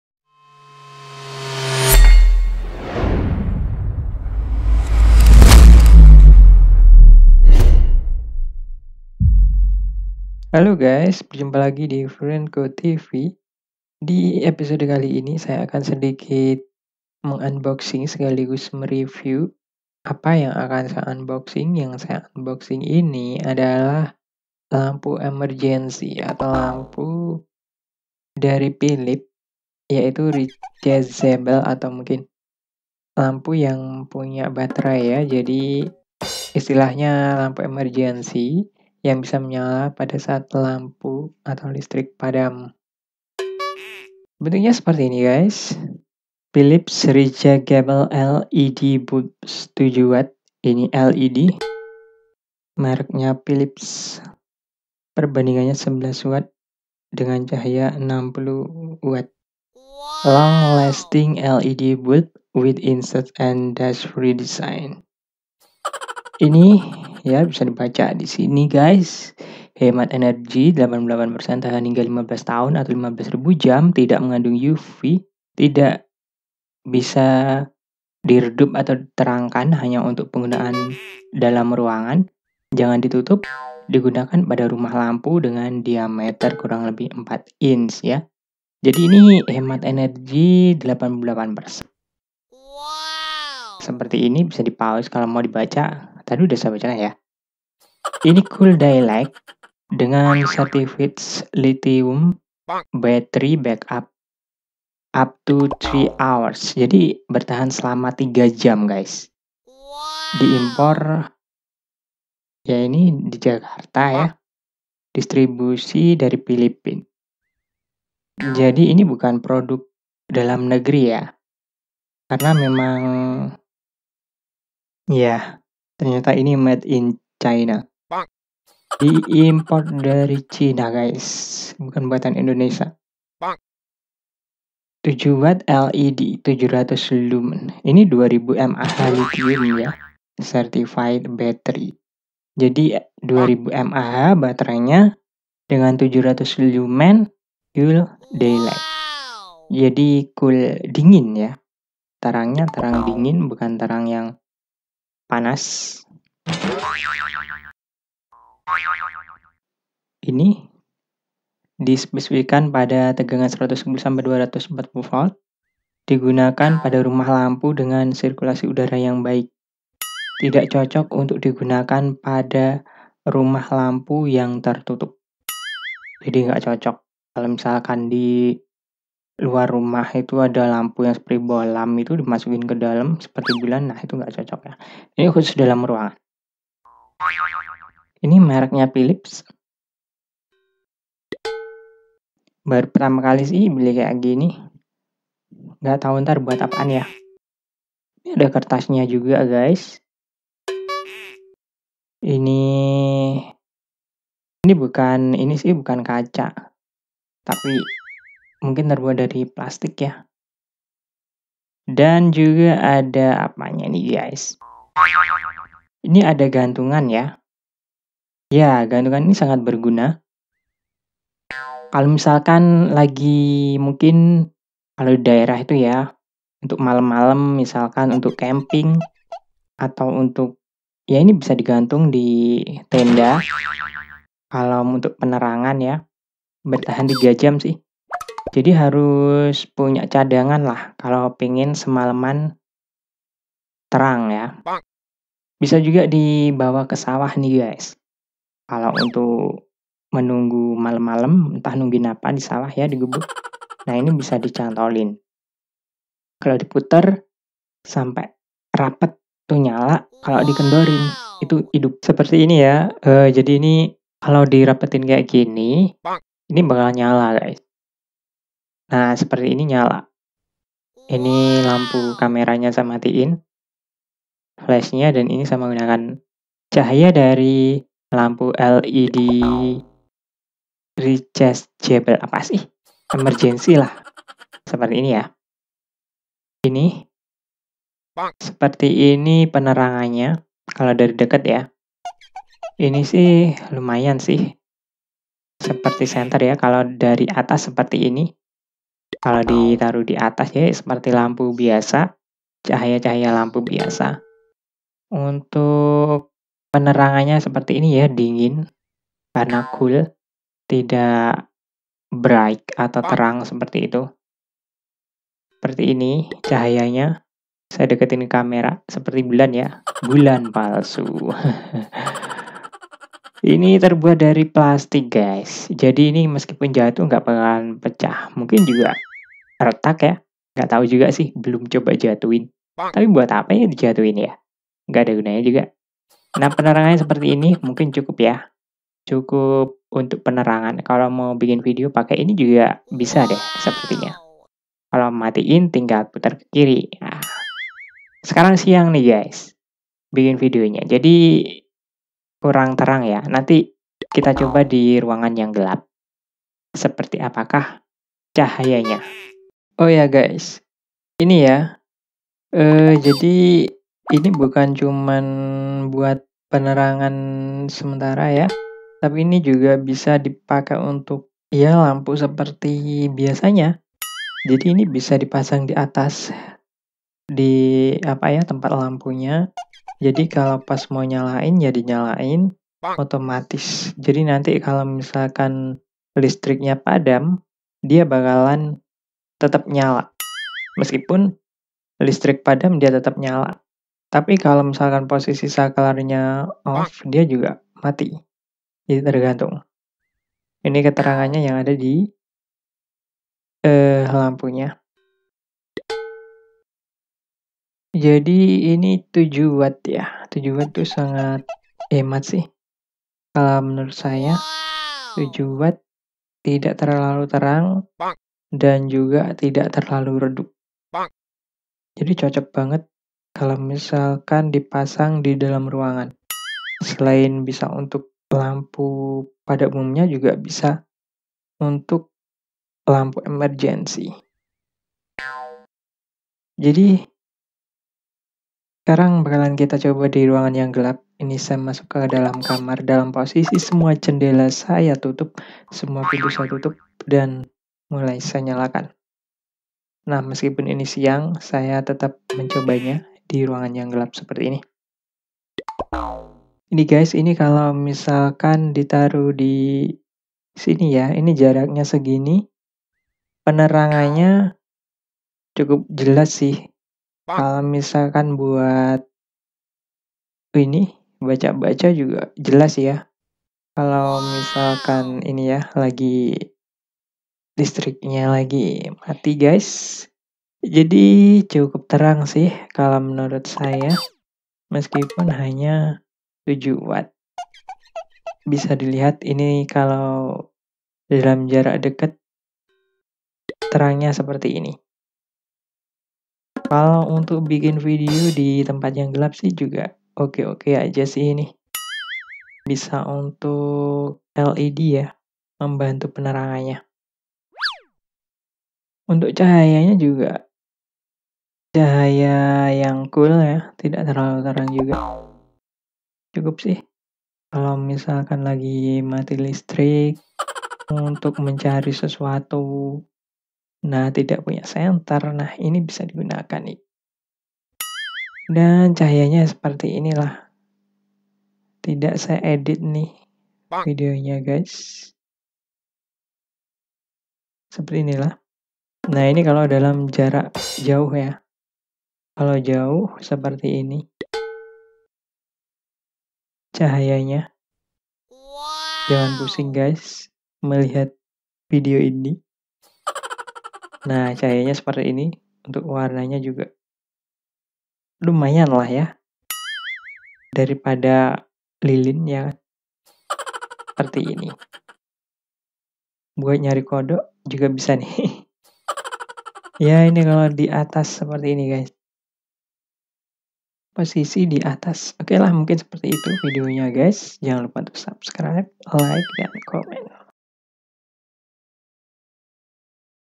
Lampu Led Emergency Philips Ternyata Begini Solusi Mati Listrik Biar Lampu Tetap Terang Selama 3 Jam
Di video kali ini saya membuka dan meriview lampu led emergency philips dengan daya baterai 2000mAH yang mampu bertahan hidup kurang lebih selama 3 jam.

Lampu emergency ini harus di chas dulu sebelum dipakai untuk berpetualang, jadi beberapa jam sebelum dipakai harus terpasang pada rumah lampu dan dalam posisi nyala listriknya, agar baterai terisi harus lampu dinyalakan.